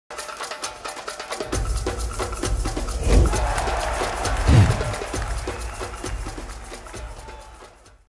CABECERA V TORNEO FÚTBOL VETERANO CIUDAD DE LA ALHAMBRA 2022
Cámara: Antonio J. Gómez Malagón - UAS Aerosistemas. Careta de entrada para el evento creada por nuestro equipo de diseño.